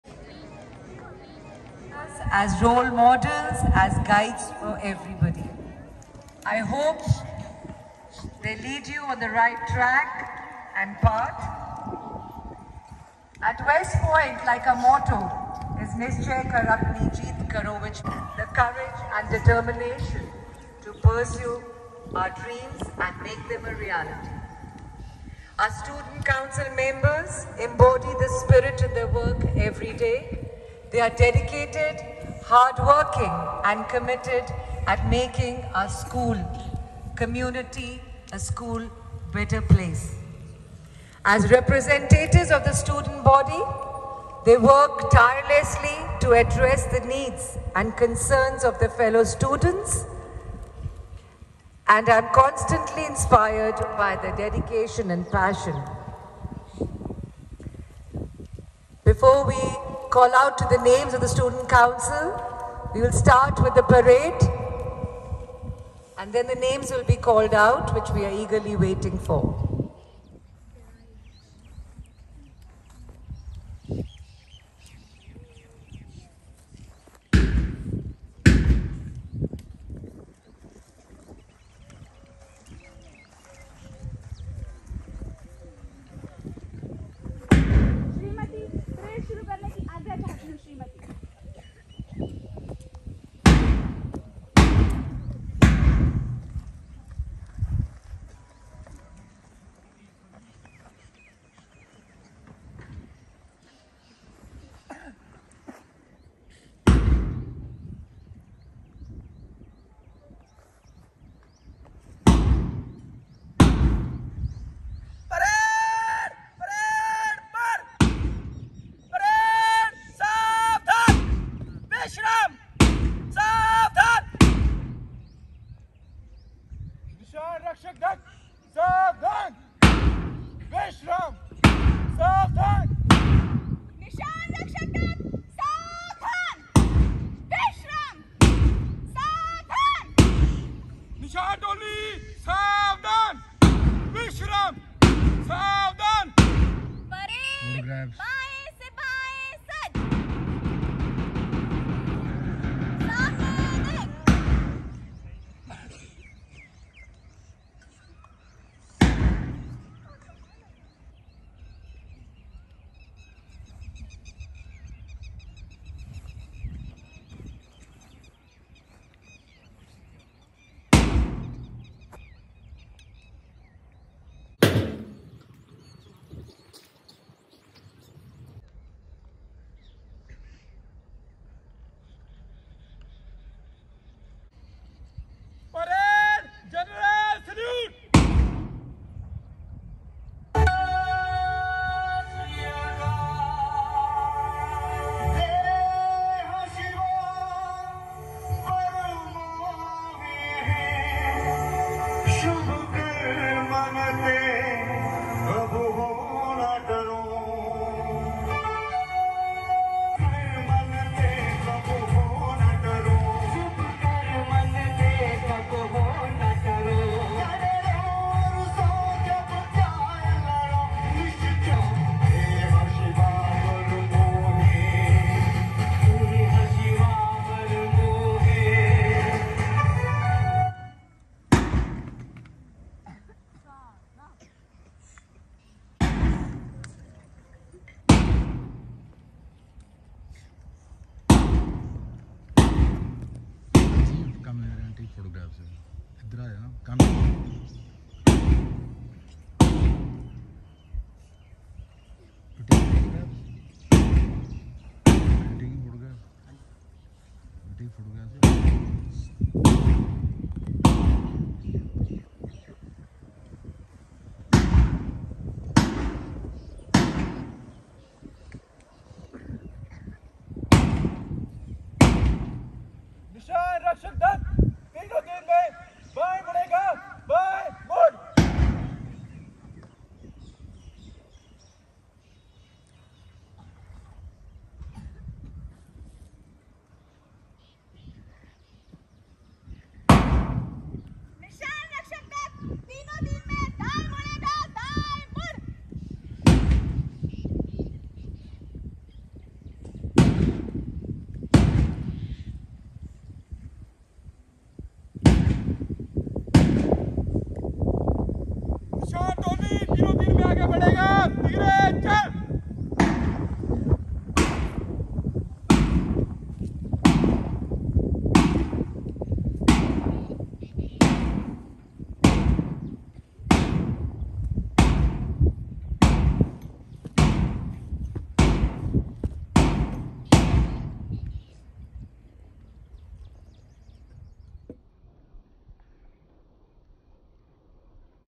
WEST POINT THE MAJESTIC SCHOOL Investiture ceremony , formation of the student council class 12th .
WEST POINT THE MAJESTIC SCHOOL Investiture ceremony , formation of the student council class 12th . March past trooping of the school Flag.